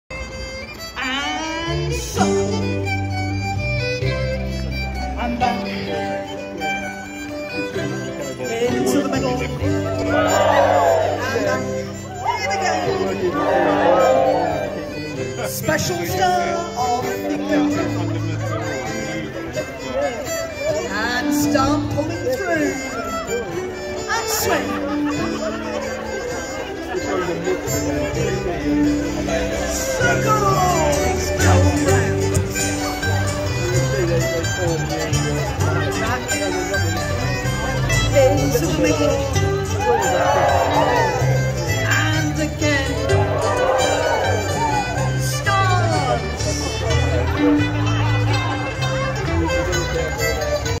Ceilidh, Folk Weekend On Tour Faringdon 2021, The Little Big Canal Dance Band
Ceilidh in Faringdon Market Place as part of the Folk Weekend On Tour Faringdon (from Folk Weekend Oxford) Saturday 9th October 2021..

Music performed by The Little Big Canal Dance Band, and the caller is Jane Bird.